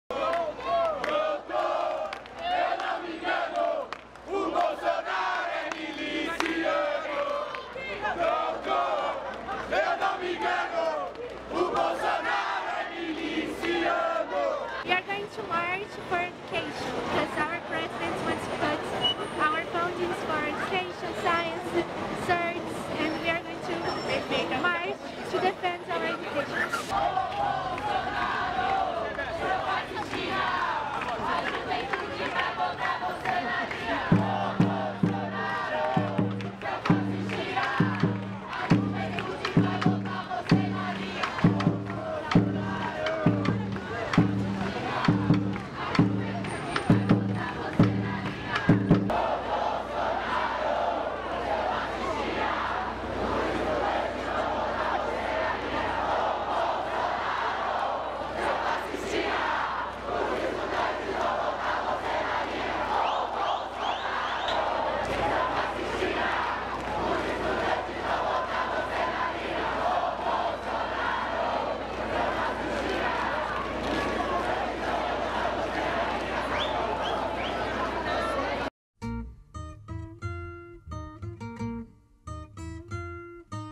Sao Paulo Teacher and Student Demo Against Bolsonaro Cuts
When Barry and I left the Sao Paulo Museum of Art we encountered student, teacher, and Professors beginning a demonstration against Brazilian President Bolsonaro’s cuts education.